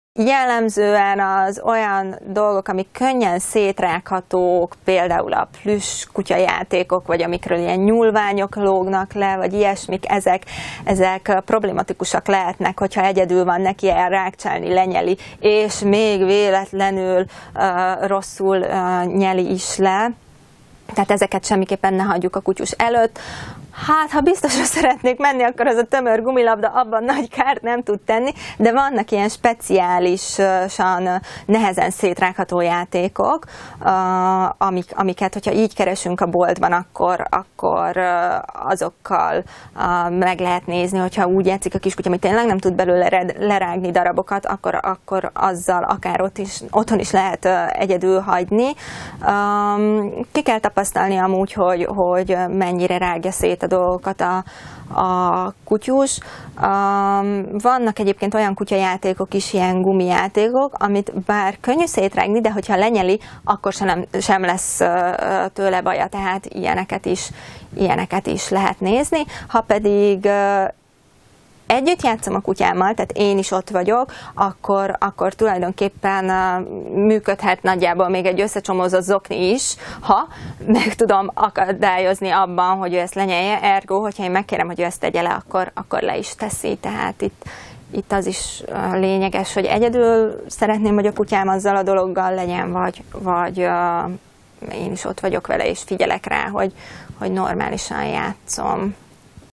Milyen játékot válasszunk a kutyánknak?
A plüss állat vagy a gumilabda a megfelelő játék? Ez attól is függ hogy egyedül játszik a kutyus, vagy mi is játszunk vele.  Száraz Kata kutyatréner válaszol, hogy melyik a legmegfelelőbb és legbiztonságosabb játék a kutyánk számára.